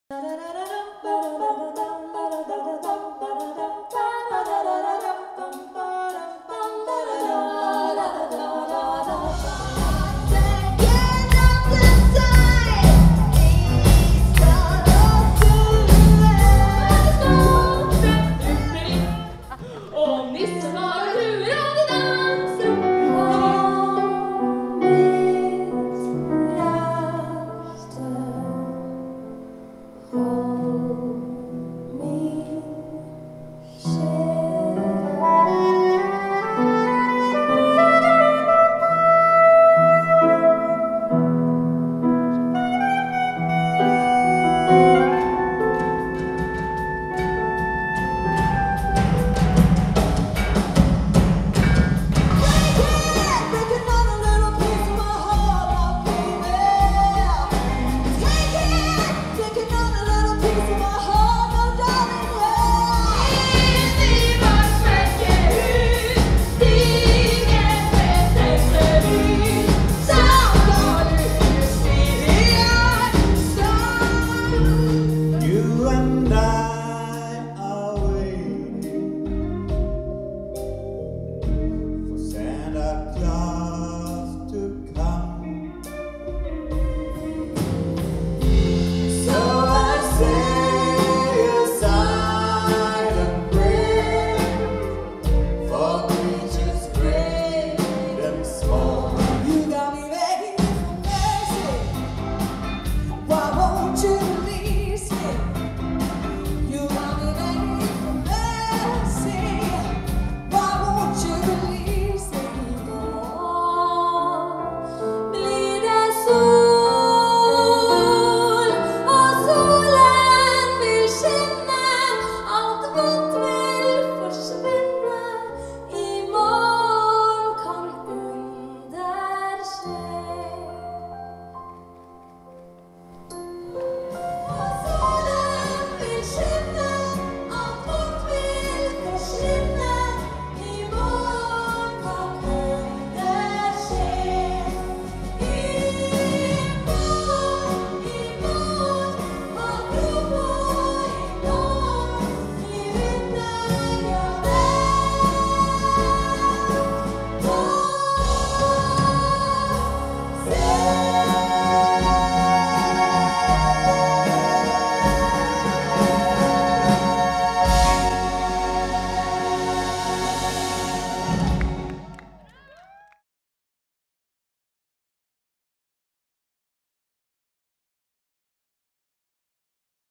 Julekava promo - SHOW MUSIKK LATTER TÅRER SMIL HUMØR - videoasbot
Juleshow 4., 5., 11., og 12. desember 2015 i Operahuset Nordfjord, med Laila Beathe Førde, Hege K. L. Naustdal og Grete Berntzen i front.